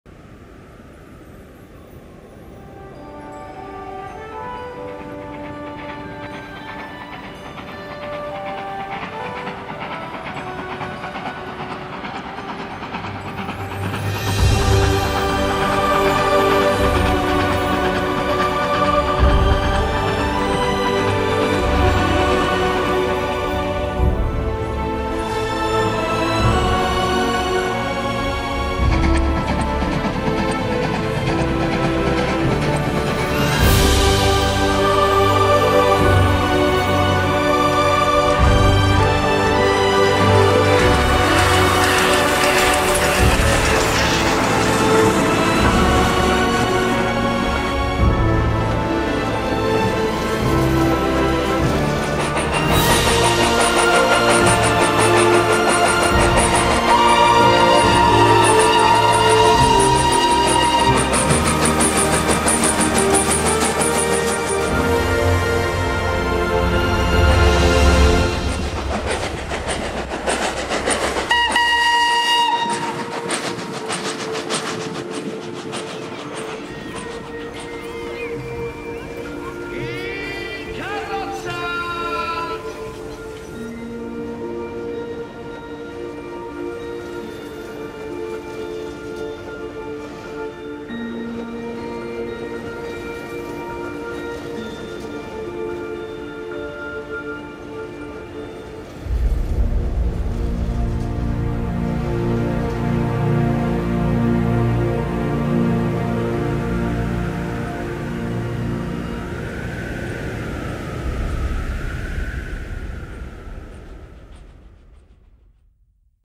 Il Polar Tanaro Express - Treno Storico sulla Ceva-Ormea
"Un consiglio sui treni: l'importante non è dove vanno, l'importante è decidere di prenderli".
La Valle Tanaro di certo ha deciso di non perderlo il treno. Ogni anno, l'8 di Dicembre, una locomotiva a vapore guida il treno storico lungo la linea di 36km da Ceva ad Ormea. Quest'anno l'atmosfera è resa ancora più fiabesca dalla neve, che fa assomigliare il treno al fantastico convoglio del romanzo "The Polar Express".
L'augurio è quello di "crederci veramente", cercare di sfruttare il più possibile la ferrovia che tanto ha dato e tanto ancora può dare a queste valli, sia dal punto di vista turistico, che di quotidiana mobilità sostenibile.